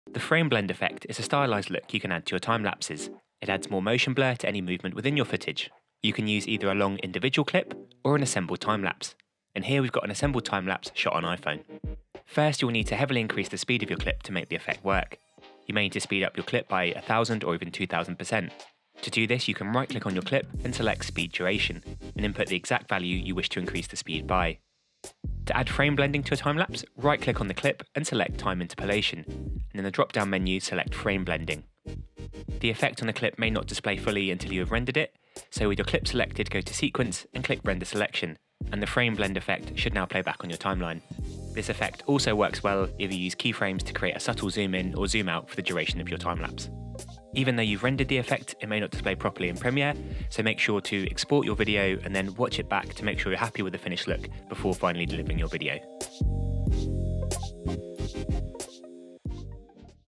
Frame Blend Timelapse Efffect in Premiere Pro
In this Sixty Second Tutorial we look at the stylized frame blending effect that you can add to your timelapses in Premiere Pro. We cover the following:

1. The frame blend effect is a stylised look you can add to your time-lapses. It adds more motion blur to any movement within your footage.

2. You can use either a long individual clip or an assembled timelpase and here we’ve got an assembled timelapse shot on iPhone.

3. First you will need to heavily increase in the speed of your clip to make the effect work, you may need to speed up your clip by 1000 or even 2000%.
To do this you can right click on your clip and select ‘Speed Duration’ and input the exact value you wish to increase the speed by.

4. To add frame blending to your timelapse right click on the clip and select ‘Time Interpolation’ and in the drop down menu select frame blending.

5. The effect may not display until you have rendered it, so with your clip selected go to ‘Sequence’ - ‘Render Selection’ and the frame blend effect should now play back.

6. This effect also works well if you use keyframes to create a subtle zoom in or zoom out for the duration of the timelapse.